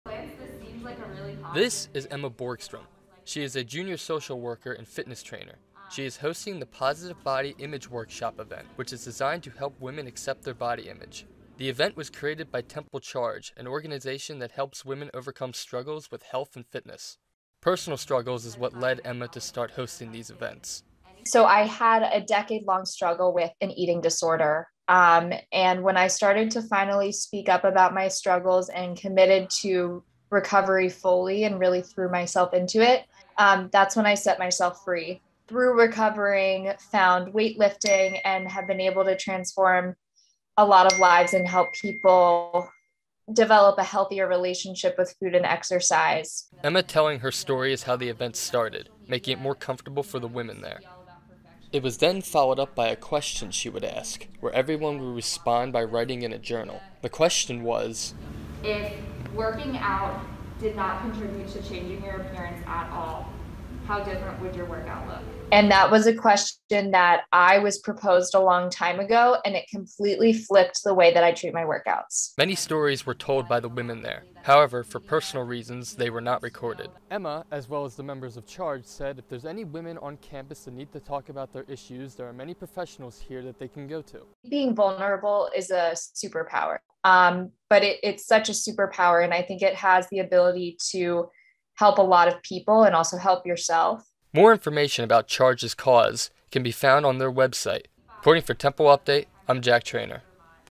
Positive Body Event Helping Women on Campus
Temple Update's Jack Traynor is reporting on Temple's Positive Body Image Workshop Event.